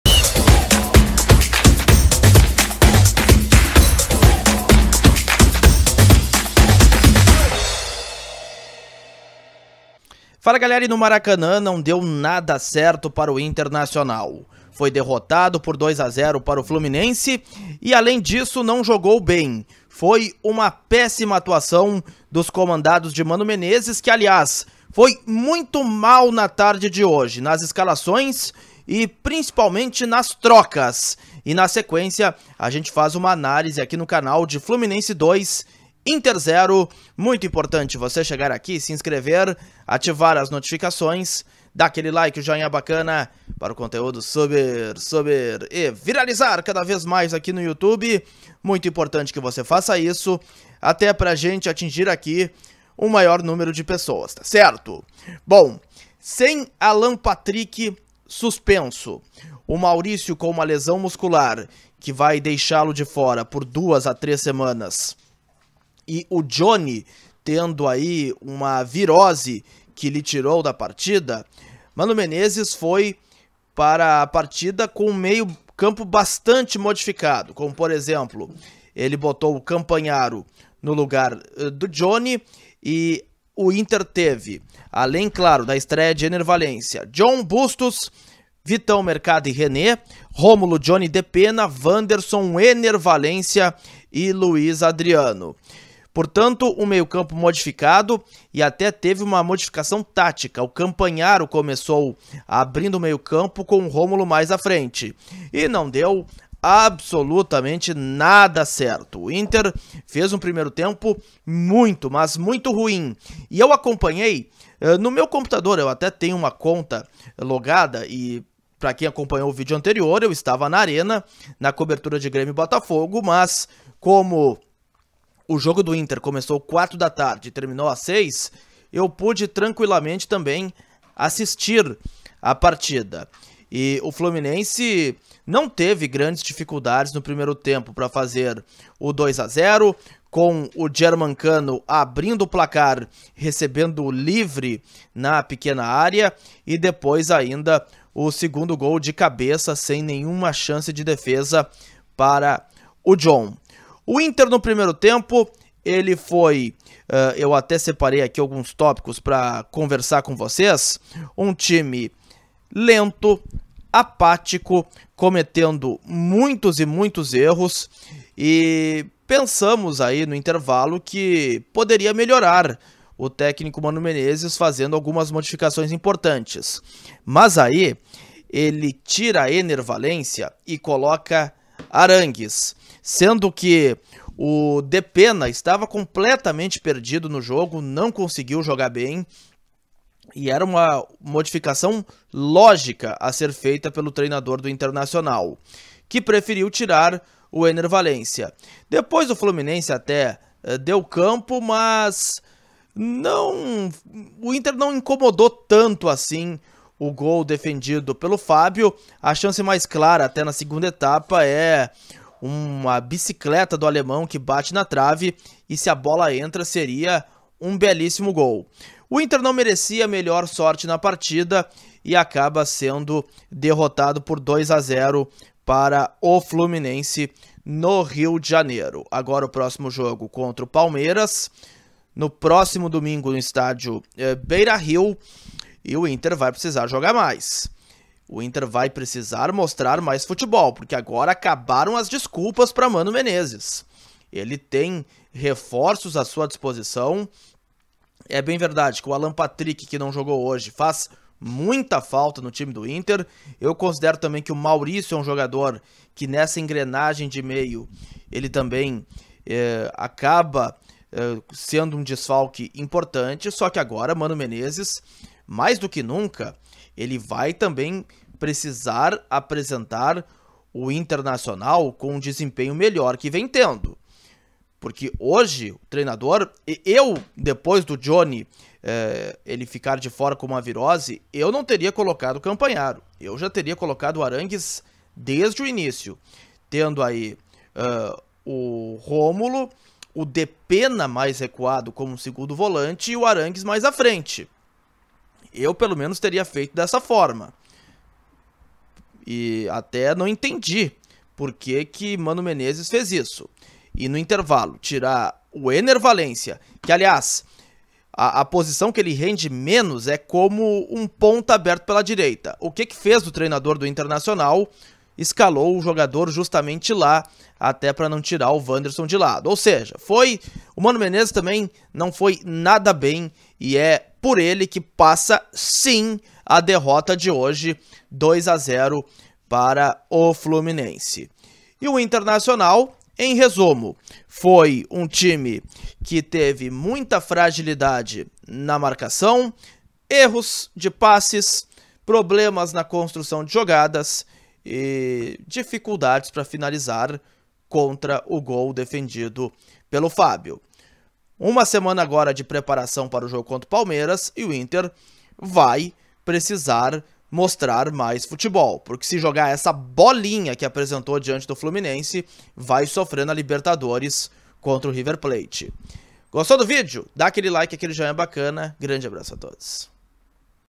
FLUMINENSE 2 x 0 INTER | COLORADO NÃO JOGOU NADA | MANO ERROU TUDO | A ESTREIA DE ENNER VALENCIA
Confira a análise do jogo no vídeo!